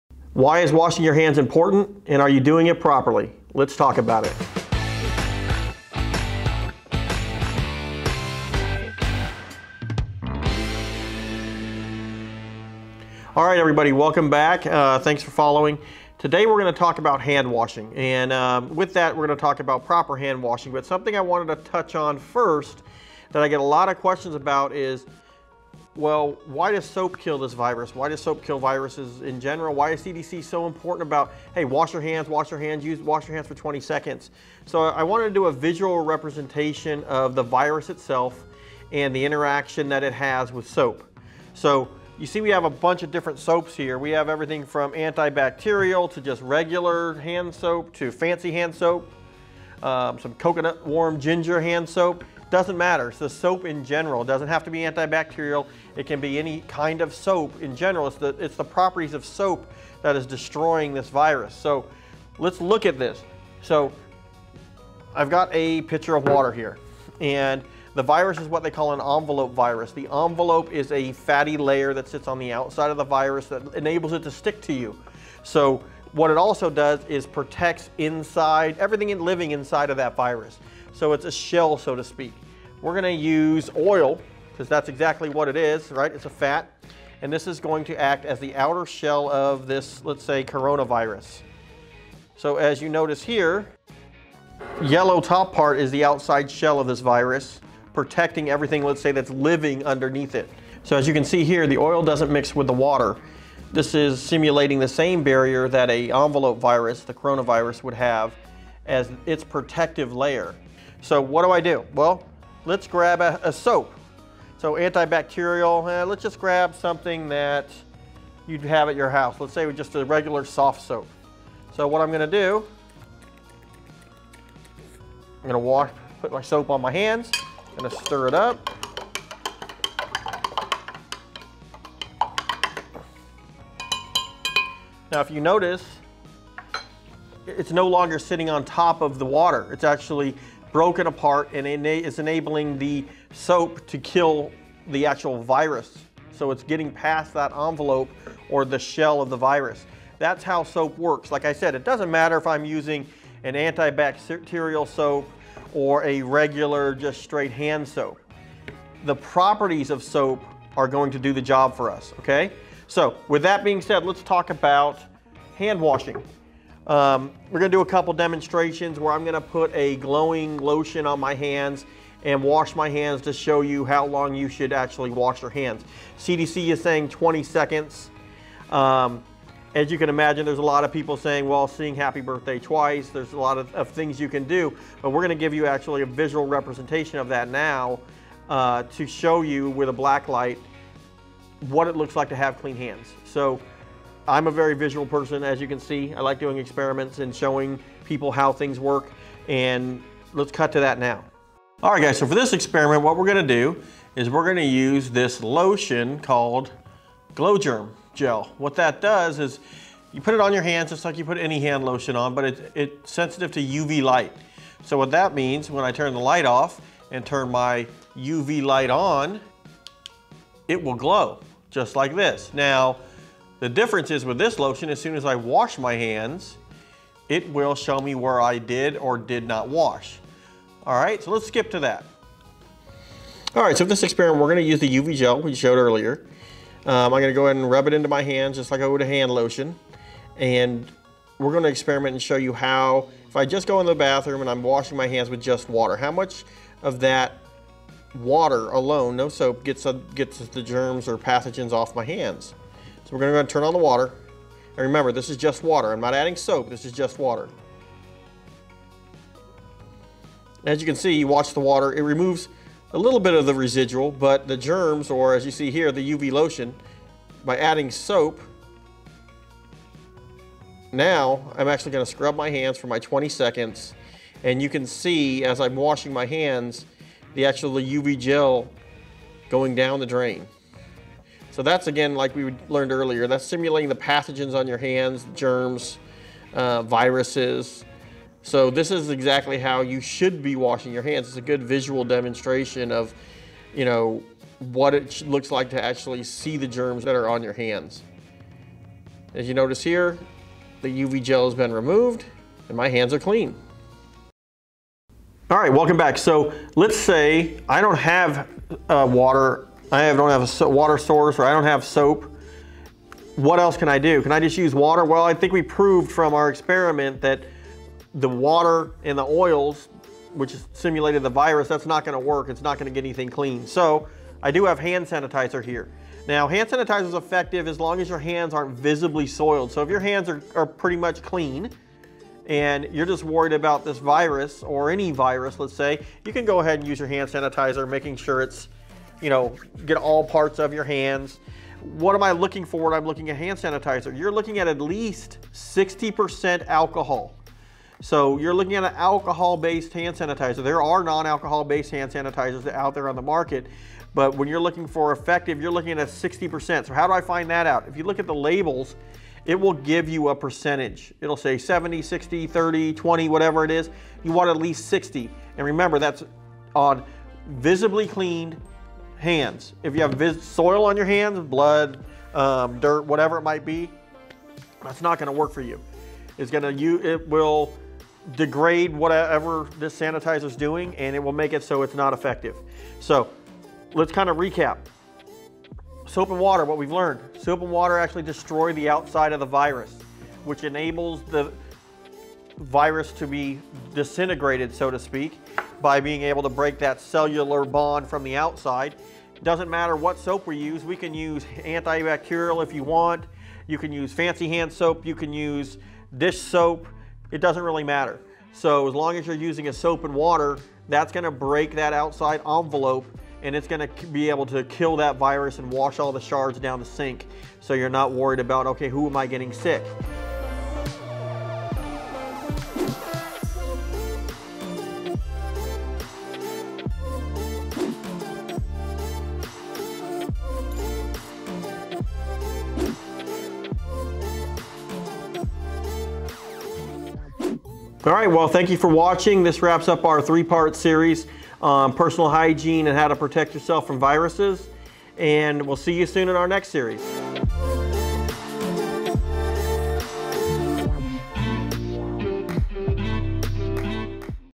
How Handwashing Kills Viruses | Train With Us
We've all been told to wash our hands with soap for at least 20 seconds, but do you know why? In this episode of Train With Us, we demonstrate how soap destroys envelope viruses like COVID-19. We also demonstrate why 20 seconds is necessary.